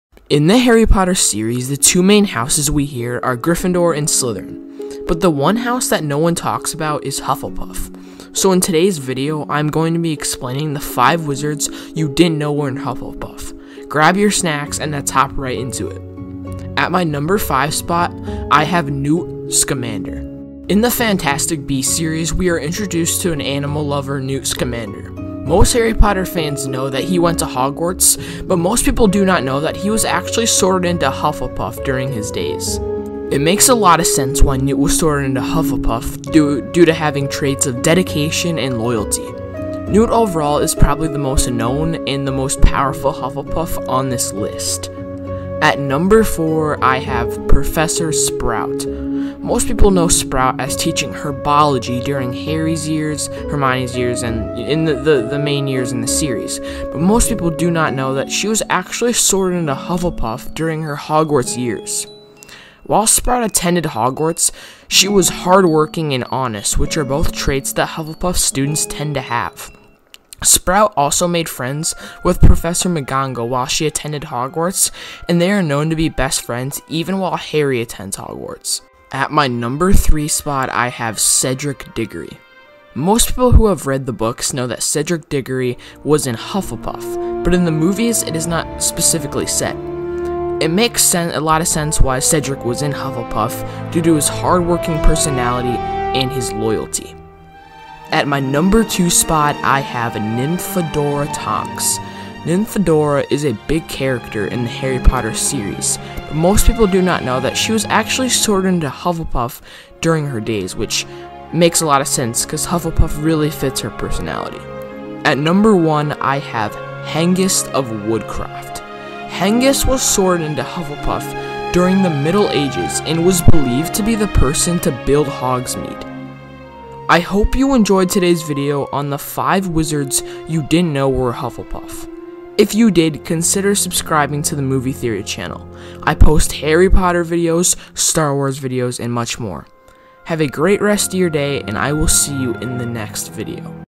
5 Wizards you Didn't know were Hufflepuff
In this video I explain 5 Wizards you Didn't know were Hufflepuff